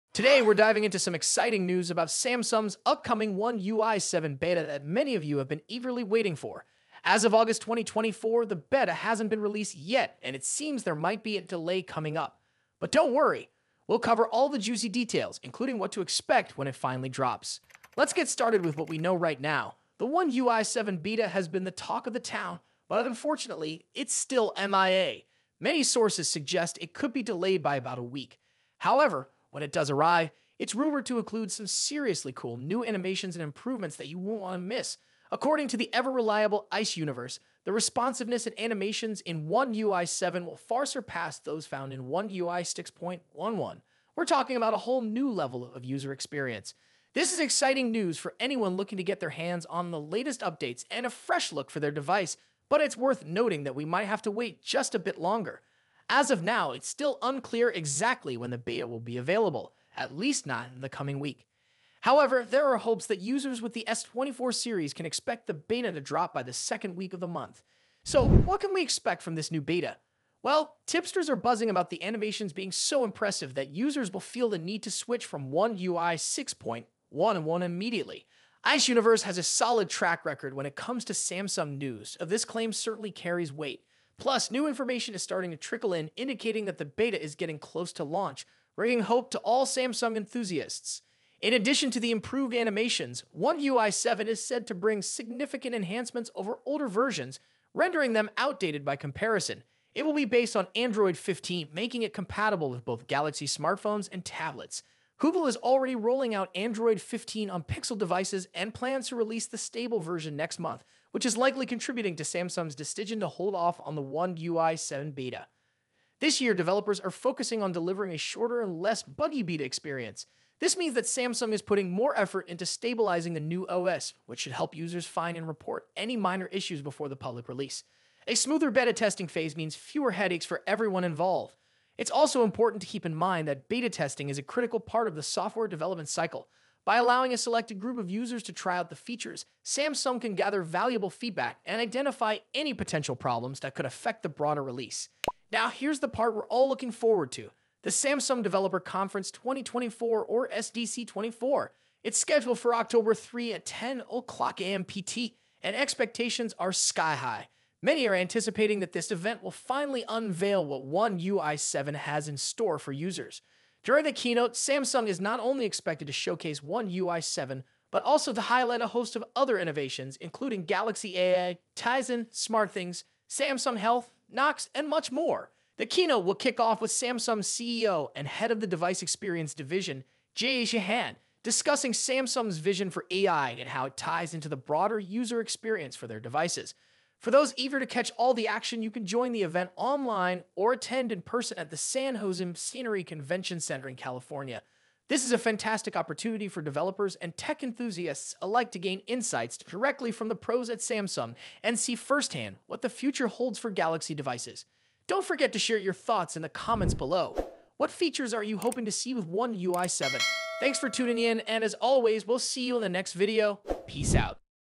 Will the One UI 7 Beta Include New Animations? Find Out!
The Samsung One UI 7 beta has been delayed, but don’t worry—there’s plenty to look forward to! In this video, we cover everything you need to know about the upcoming One UI 7 beta, including its exciting new animations, improved user experience, and key features that set it apart from previous versions. Samsung’s One UI 7 beta, based on Android 15, promises to deliver significant improvements that are sure to impress Galaxy users. Though the release has been delayed, rumors suggest we won’t have to wait long before it drops. Get all the insider details and be prepared for what’s coming next! Plus, learn about the Samsung Developer Conference 2024 and what it could reveal about Samsung's latest innovations.

Don’t miss out—watch now and get ahead of the game!